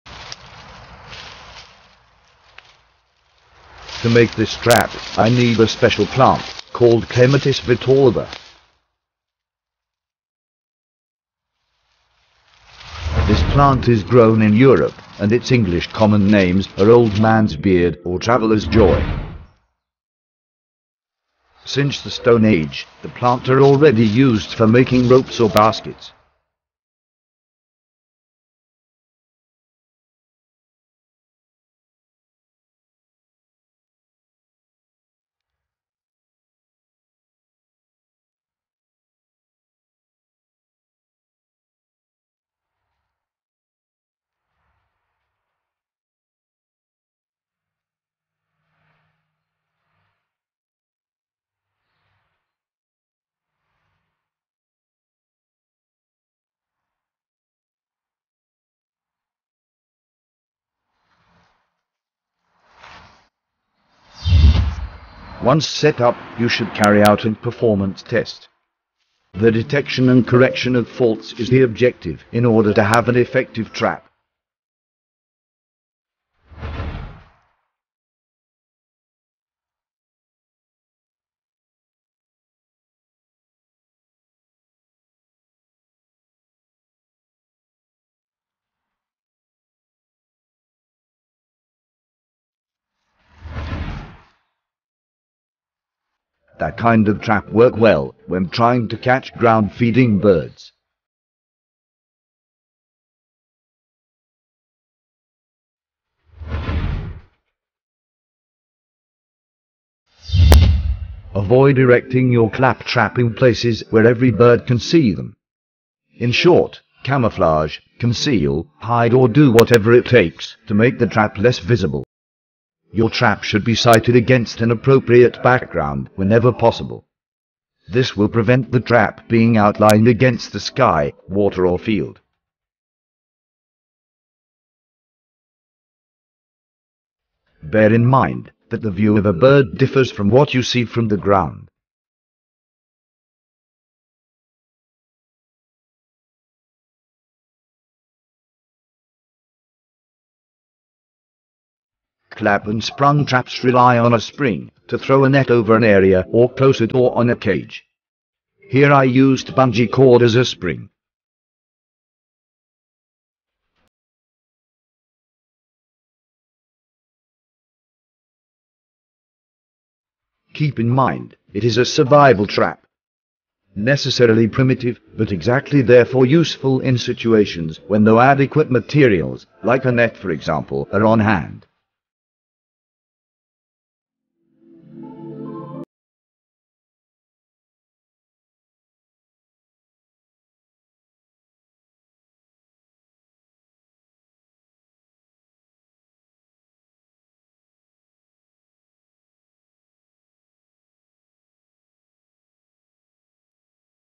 Survival clap trap (Bird trap)
How to build a primitive survival trap to catch ground-feeding birds.
Required materials: bungee-cord (3x), a knife and some natural materials.